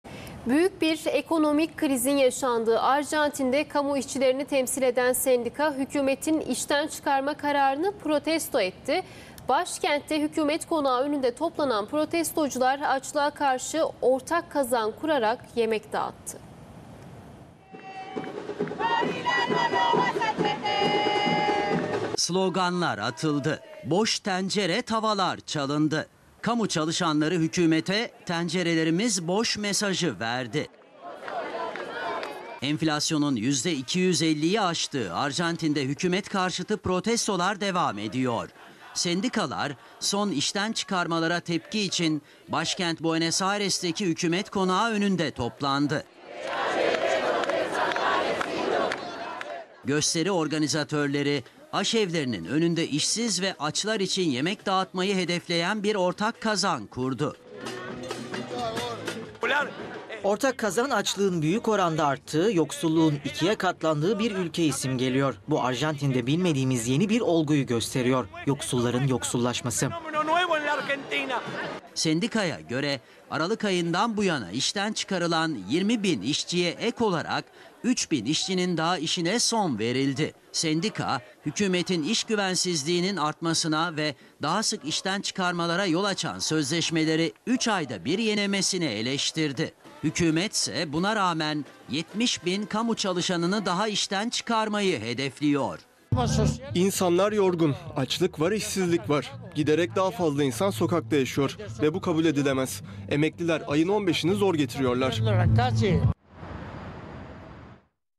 Arjantin'de İşten Atılanlar İsyanda! İşçiler Sokağa Döküldü: Geçinemiyoruz!
Arjantin'de işçiler "Geçinemiyoruz" diyerek sokağa döküldü.

En son haberler, en güncel gelişmeler ve uzman yorumcuların haber analizleri CNN TÜRK ekranında canlı yayınlar ile izleyicisiyle buluşuyor.